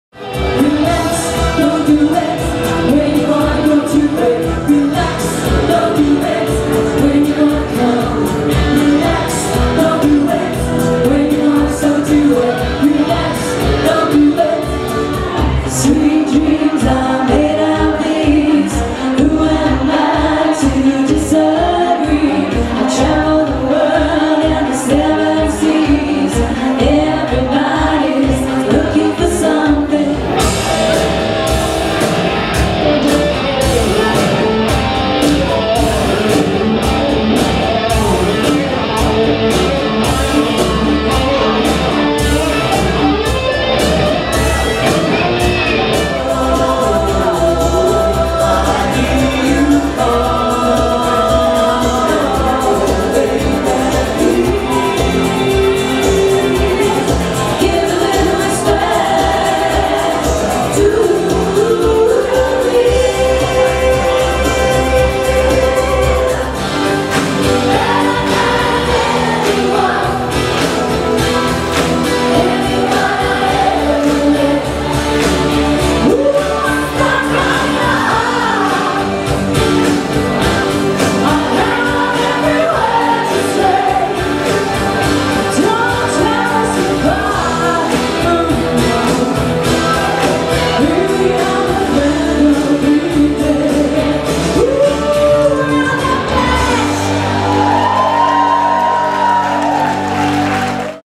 The 80's Tribute Show - Live Performance Show Reel AliveNetwork.com
A fun packed show featuring classic songs from the 80s! With exceptionally talented vocalists & musicians, The 80s Show includes authentic costumes, amazing choreographed numbers & tons of audience participation!